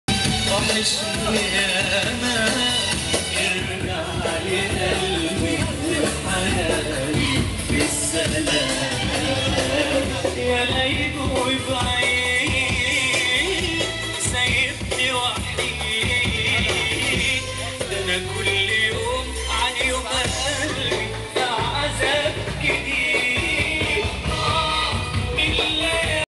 Oriental Dance. Egypt. Resort of Sharm el Sheikh | Rest and Travel
Egyptian dance ensemble performance.
On the square in the old town of Sharm El Sheikh. Circle is bazaar and place for leisure travelers. Dancers Egyptian man and girl dancers perform Oriental dance.

Egyptian dance. Egypt. Resort of Sharm el Sheikh